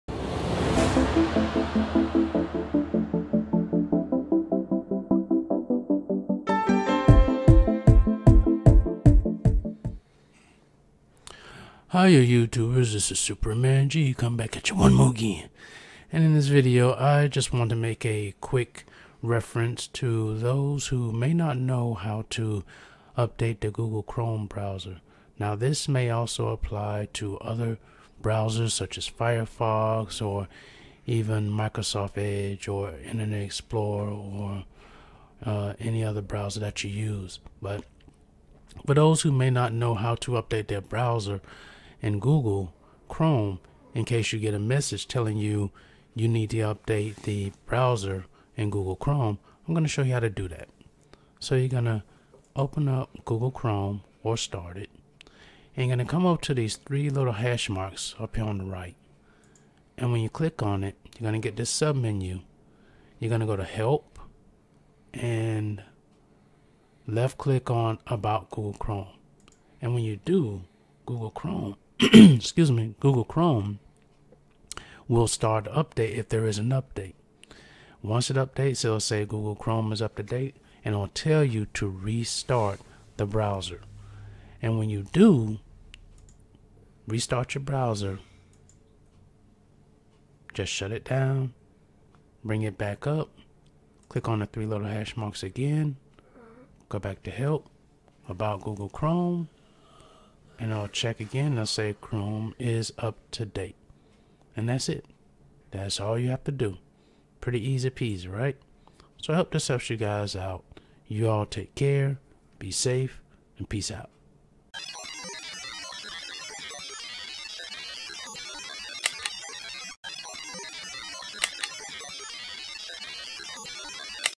How To Update Google Chrome
In this short tip, I show you how to update Google Chrome browser. This also applies to most other web browsers as well.